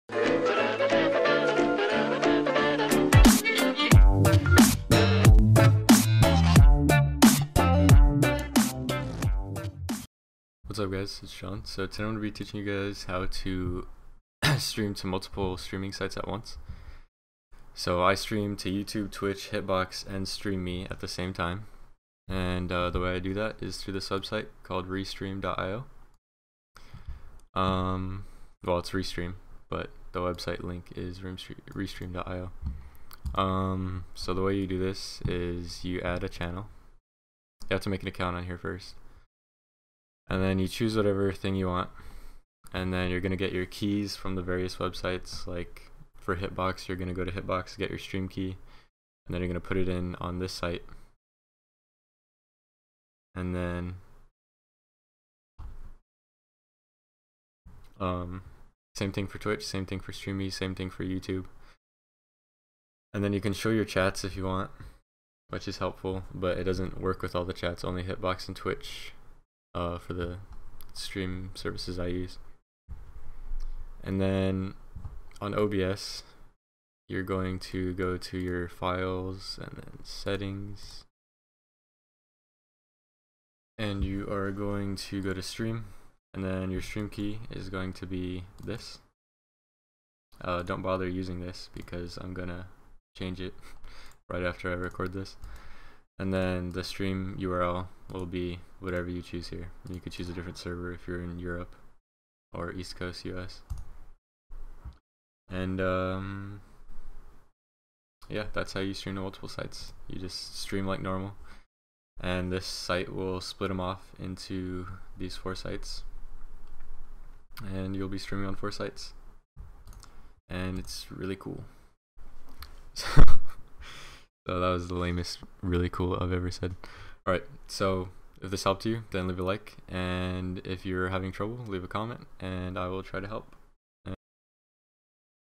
How to Stream to Multiple Streaming Sites Using Restream
To stream to Twitch, Hitbox, Stream.me, YouTube and whatever other sites you want at the same time use:
Streaming like this will not hurt the performance of your computer.